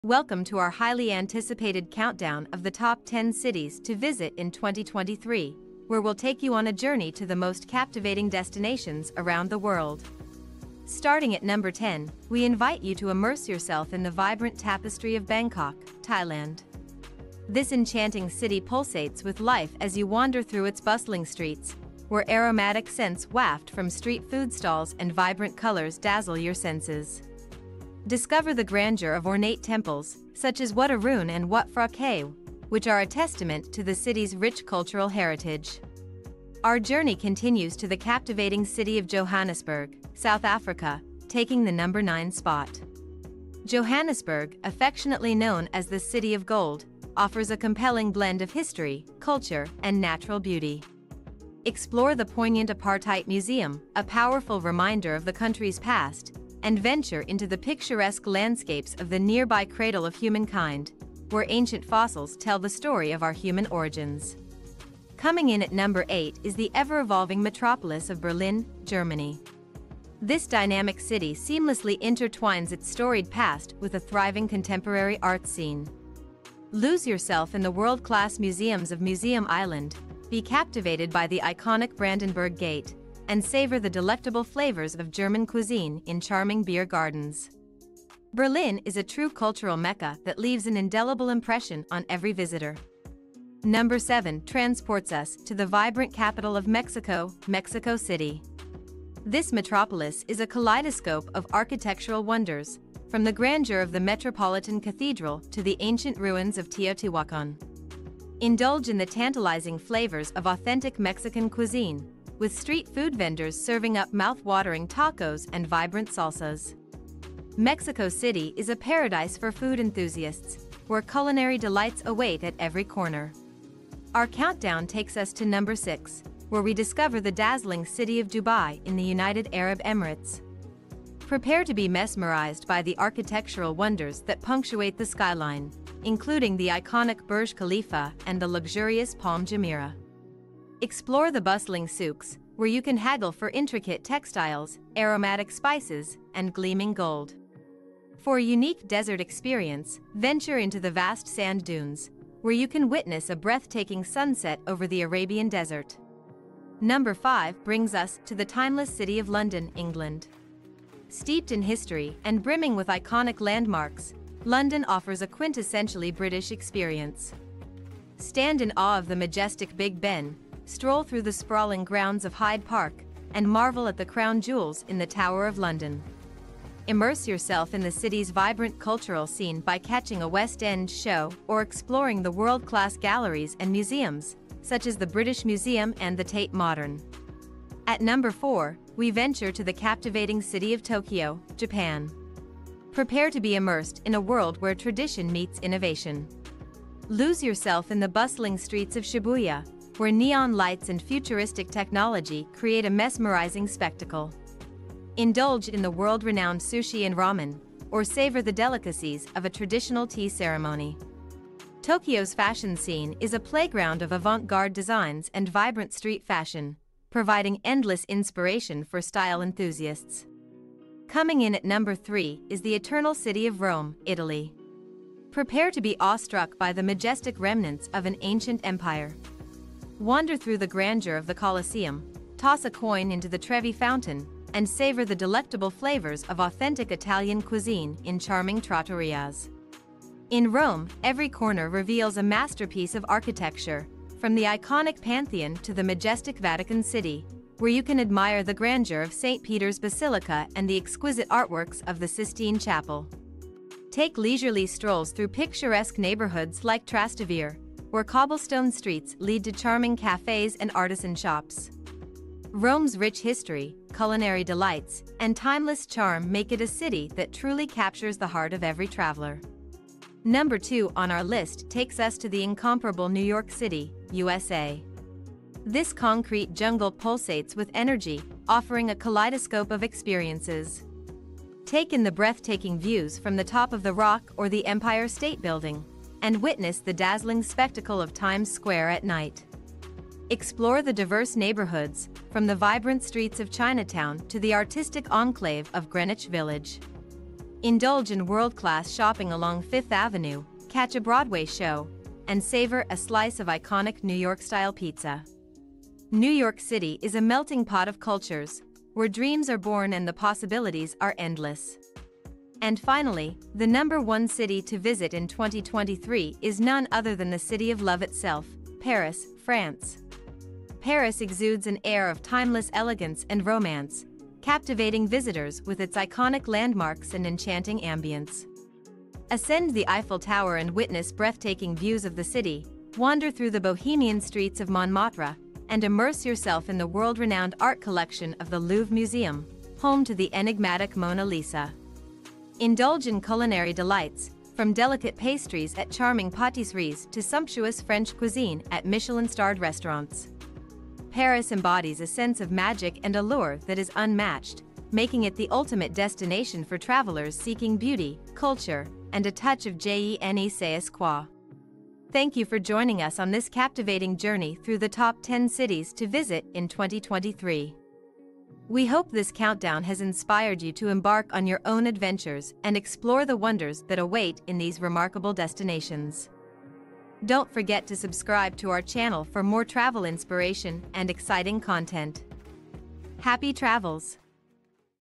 Top 10 MUST visit Cities in 2023 | Travel Video
Join us on an exhilarating journey as we count down the top 10 cities to visit in 2023! From the vibrant streets of Bangkok to the romantic charm of Paris, these destinations are sure to ignite your wanderlust and leave you longing for adventure. These destinations are a must for your bucket list.